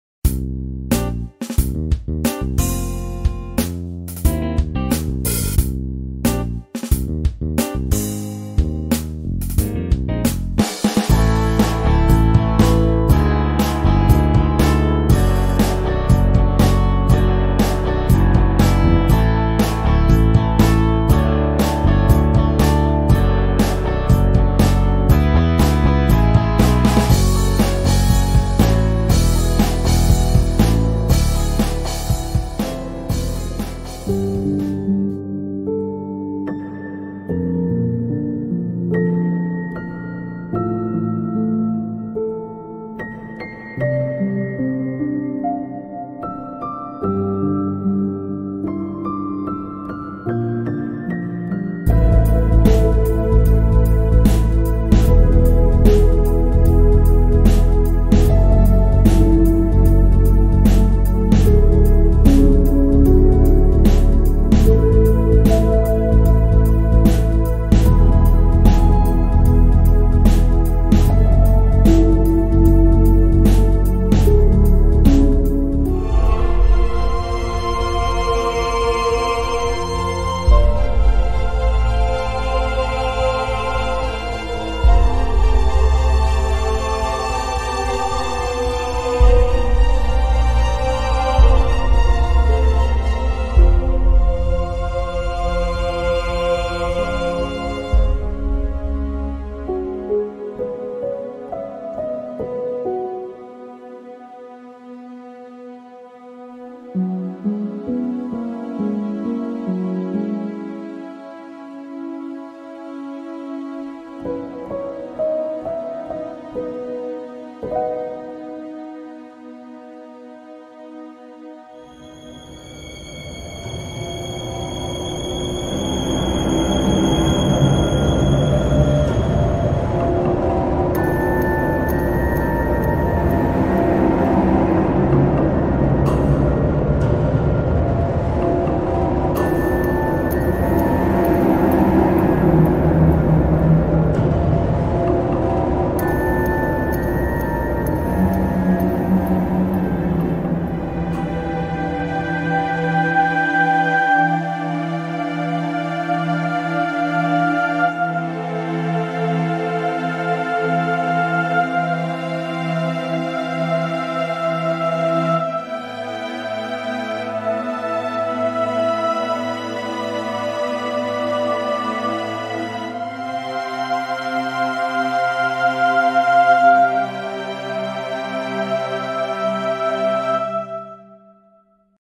GDSVideomaker - Music Reel
Here are some of the tracks I’ve written for commercials, weddings, fiction and trailers.

TIMING:
00.10 COMMERCIAL
00.52 WEDDING
1.40 FICTION & TRAILER